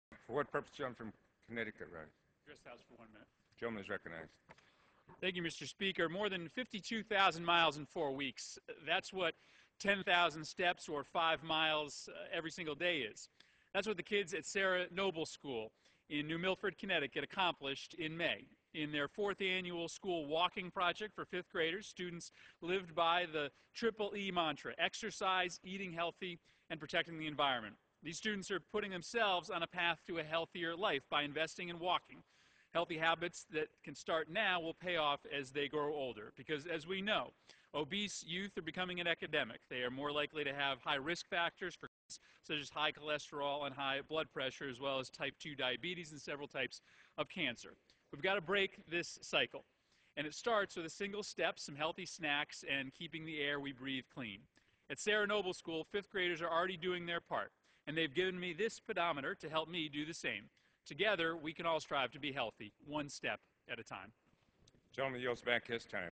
CongressmanChrisMurphyTheWalkingProject.mp4
Congressman Chris Murphy talks about the fight against Childhood Obesity while spotlighting our friends at Sarah Noble School in New Milford, CT & The Walking Project